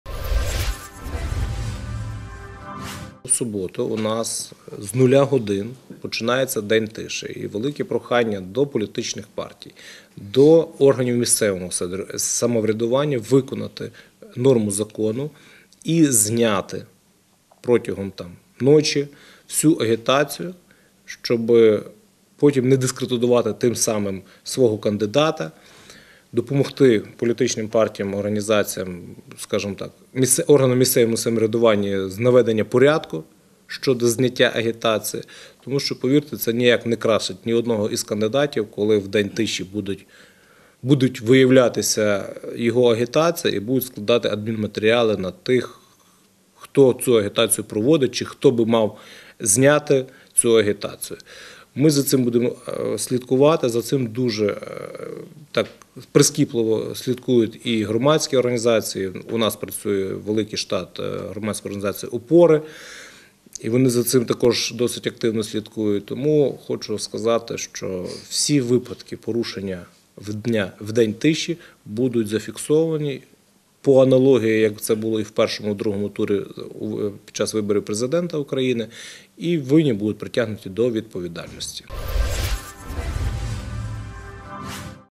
День тиші перед виборами до Верховної Ради
Поліція попереджає, що в «день тиші» перед виборами до Верховної Ради України будь-яка політична агітація буде заборонена
З 00:00 20 липня діятимуть обмеження щодо ведення передвиборчої агітації. Про це під час прес-конференції повідомив заступник начальника ГУНП в Дніпропетровській області Володимир Богоніс.
 «Поліція закликає штаби кандидатів у народні депутати України, політичні партії та органи місцевого самоврядування виконати норму закону та завчасно зняти всю виборчу рекламу. Символіка партій чи кандидатів та будь-яка передвиборча агітація буде вважатися незаконною. Поліція реагуватиме на всі випадки порушень у «день тиші» та складатиме відповідні адміністративні протоколи», - повідомив Володимир Богоніс.
Нагадуємо, що порушення встановлених законом обмежень щодо ведення передвиборної агітації, відповідно до ст. 212-10 Кодексу України про адміністративній правопорушення, тягнуть за собою  накладення штрафу на громадян від 30 до 50 неоподатковуваних мінімумів доходів громадян і на посадових осіб - від 50 до 80 неоподатковуваних мінімумів доходів громадян.